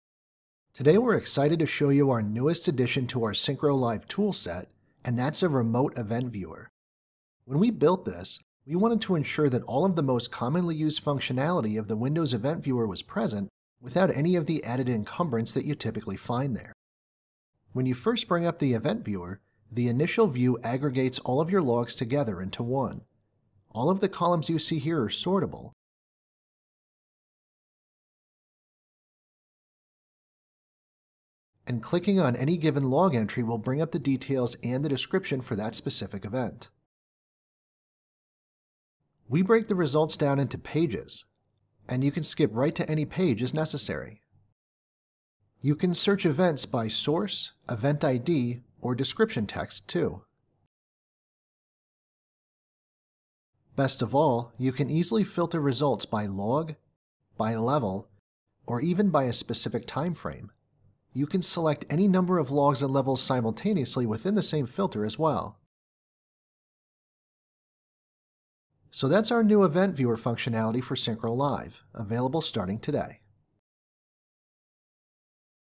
Release Update - Event Viewer Added to Syncro Live
This video covers the newly added Event Viewer within Syncro's Remote Access toolset, Syncro Live.

Now you can access, search, and filter any Asset's Event Log in the background. Watch on to learn more!

For more detailed information about Syncro Live, check out our Knowledge Base article